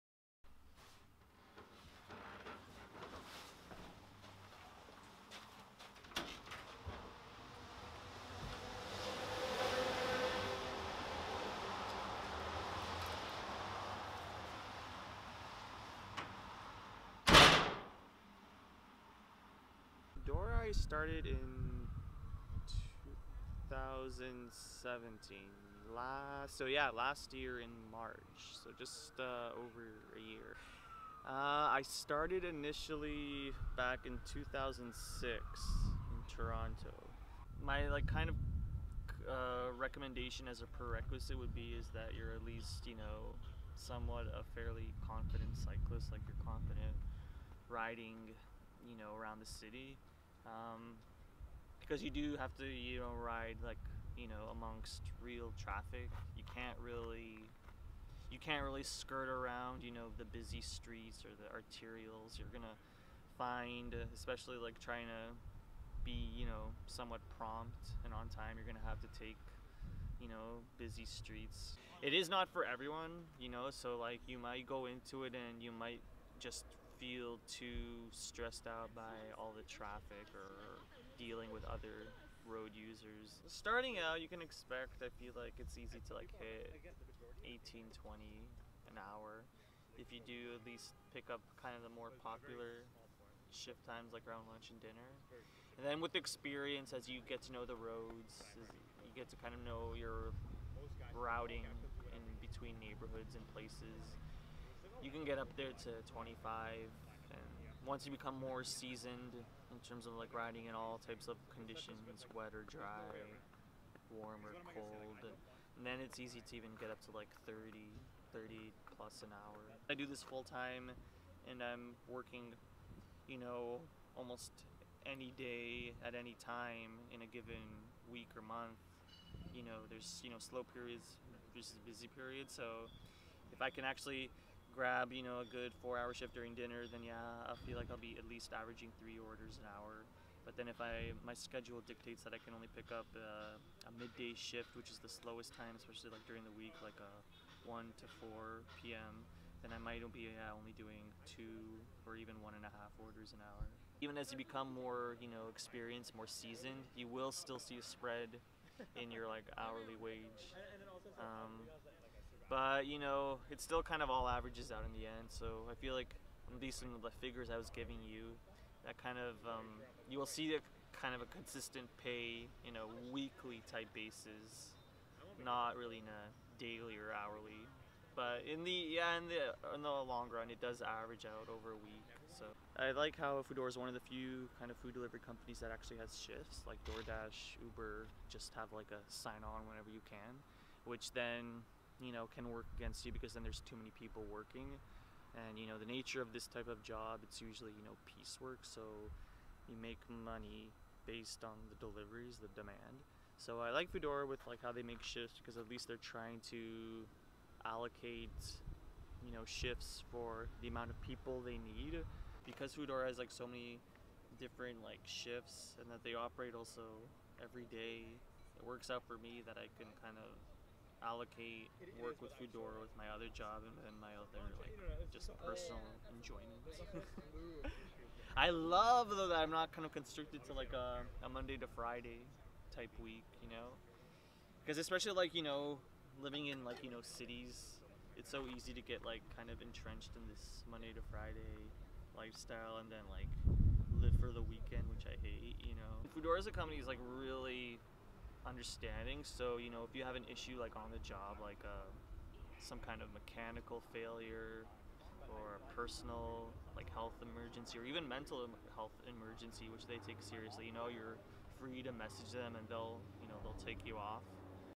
Working for Foodora
I spoke with Peter, a bike courier in Vancouver BC about his lifestyle as a full time bike courier. Here are some rather discombobulated thoughts on courier work.